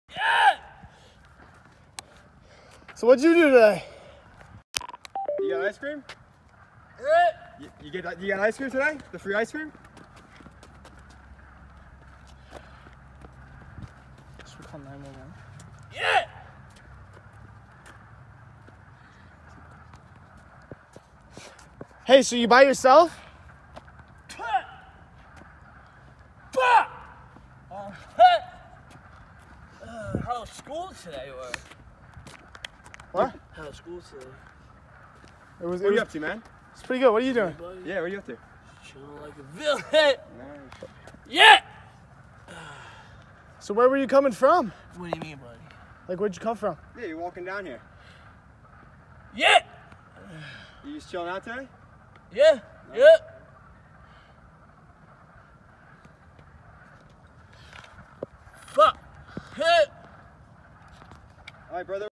How to tell if someone is on drugs
My friend and I were playing basketball and this guy encountered us that was definitely on something. If this guy isn’t on drugs then I don’t know who is 😂. We ended up calling 911 to make sure he was ok because we were more worried about the fact that we didn't know where he came from and if he was going to be fine after. This is the first time this has happened to me so I didn't know what else to do but the Police just ended up sending him home anyway. I only posted this video cause I feel like if someone acts like this in a public place, it shouldn't really be taken lightly because usually this isn't normal behavior, mental disability or not. If he had a mental disability, someone should have been watching him but instead, he was more wandering around away from his family. This video was only intended to advise, we feel bad for the guy but was more worried his health than anything else.